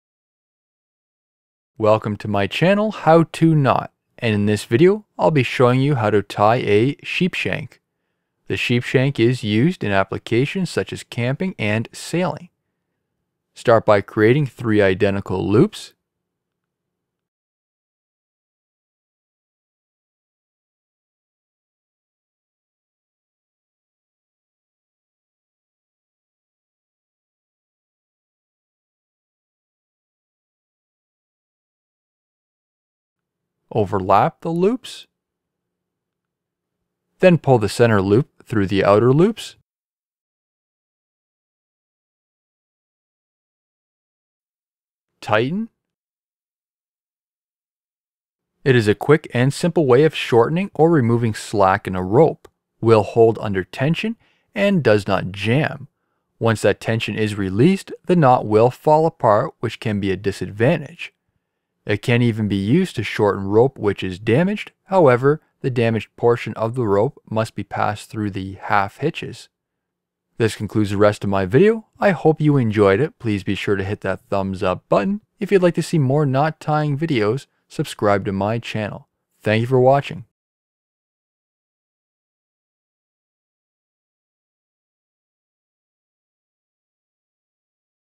How to Tie a Sheepshank
The sheepshank is used in applications such as camping and sailing. It is a quick and simple way of shortening or removing slack in a rope, will hold under tension and does not jam. Once that tension is released, the knot will fall apart which can be a disadvantage. It can even be used to shorten rope which is damaged, however the damaged portion of the rope must be passed through the half hitches.

Start by creating three identical loops.

Overlap the loops.

Then pull the center loops through the outer loops.

Tighten.